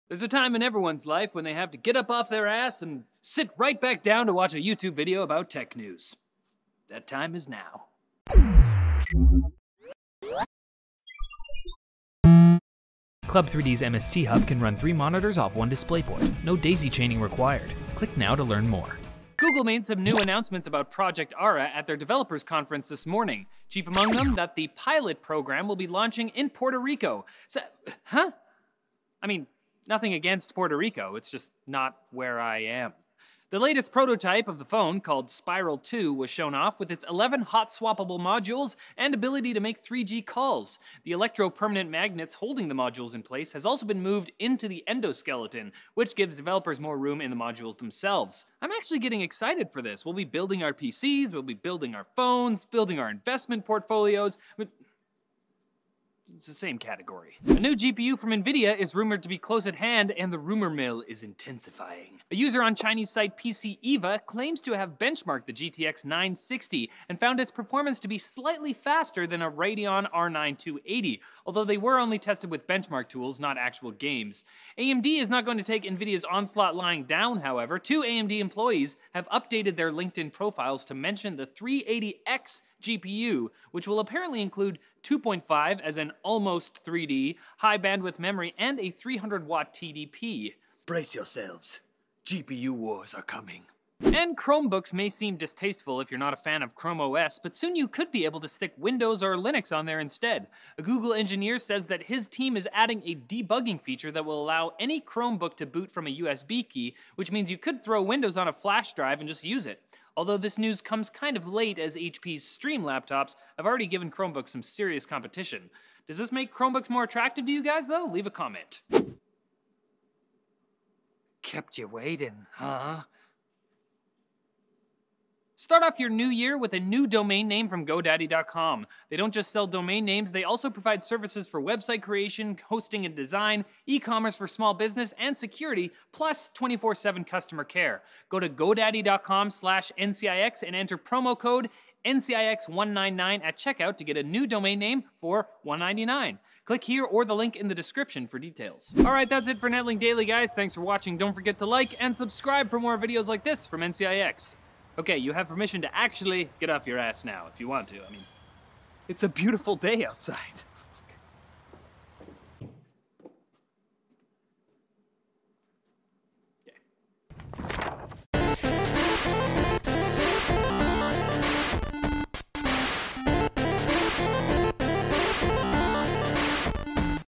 Project Ara pilot program, GTX 960 Benchmarks, Chromebooks boot from USB
Get a new .com domain from GoDaddy for 1.99 (Some restrictions apply. See site for details.)

Google reveals a new Project Ara prototype and details about the pilot program; Benchmarks and specs of the GTX 960 leak online; Chromebooks get a boot-from-usb feature.

Episode Credits:
Host: Riley Murdock
Writer: Riley Murdock
Editor: Kalvin Shum